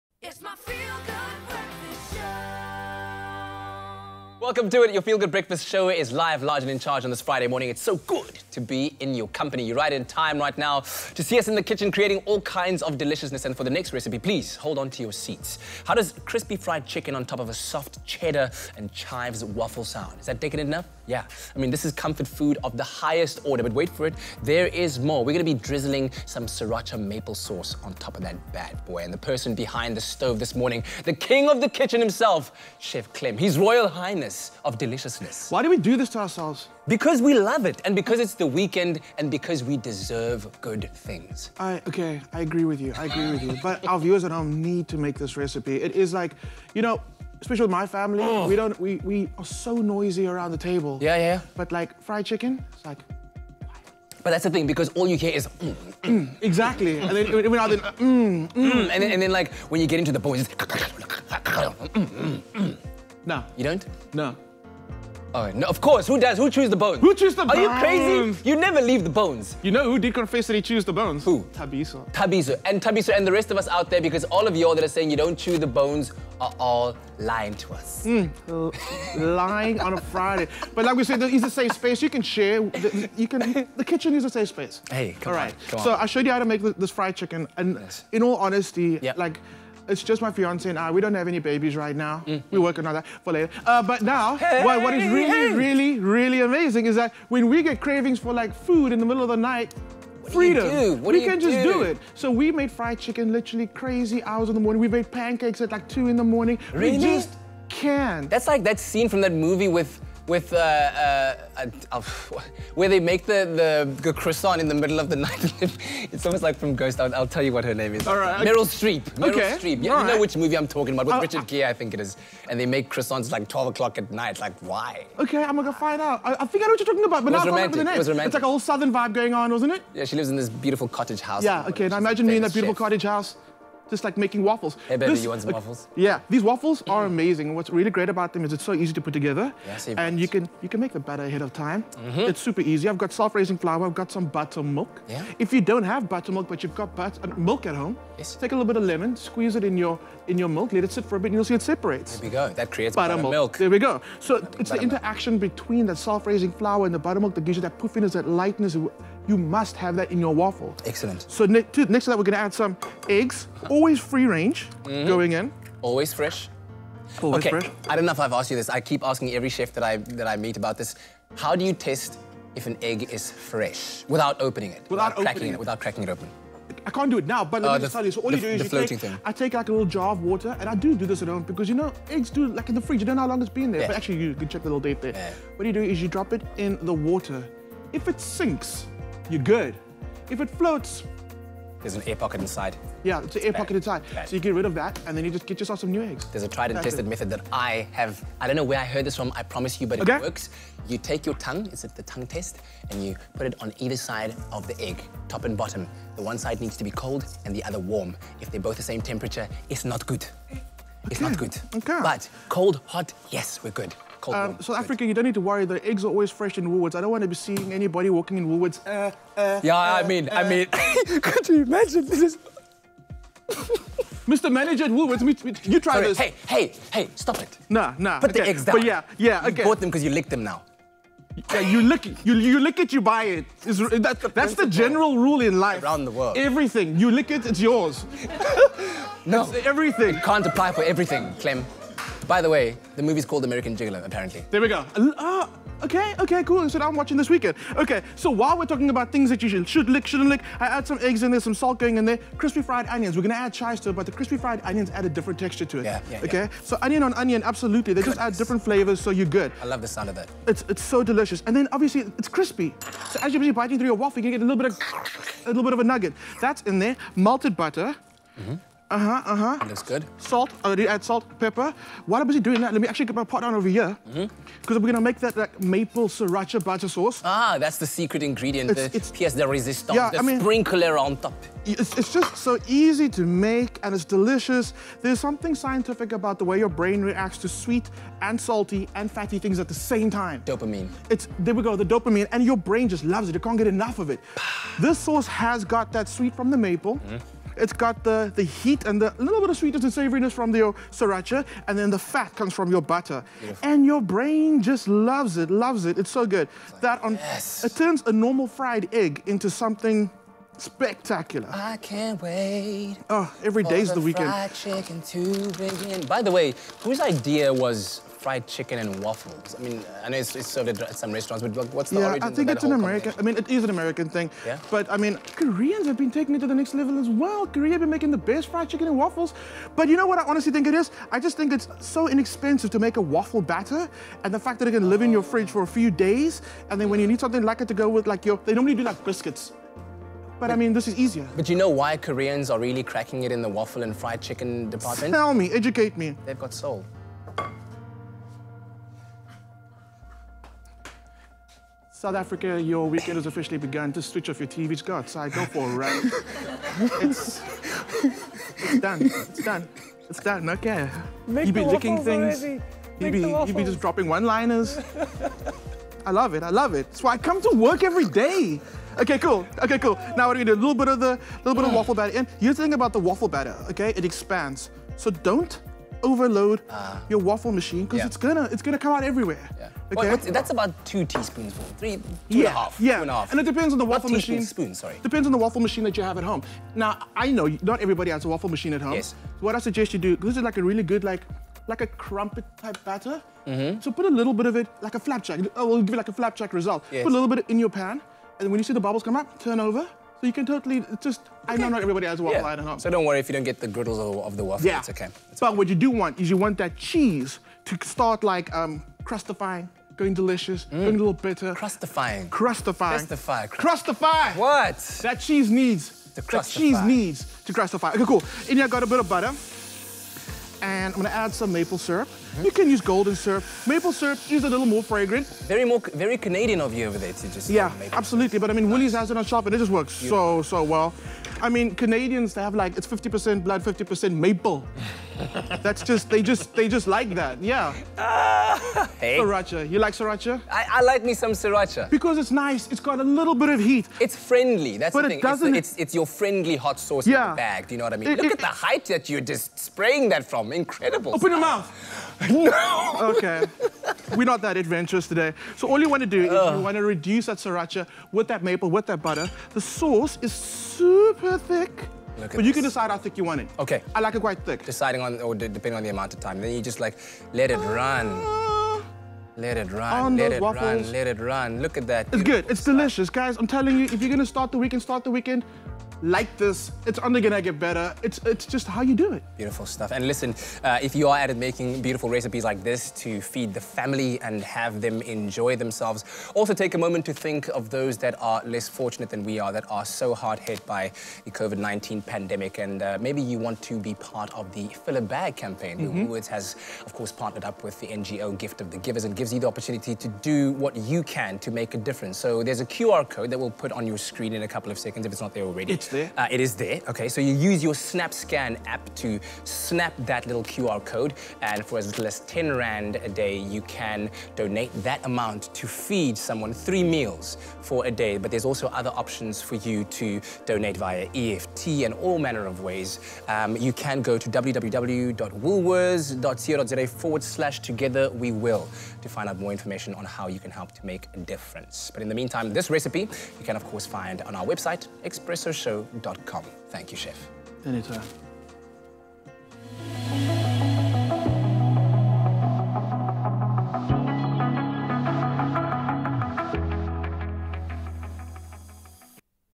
Recipe: Cheddar & Chive Waffles (WW)
Chef Clem is going to show us how to make cheddar and chive waffles topped with his crispy fried chicken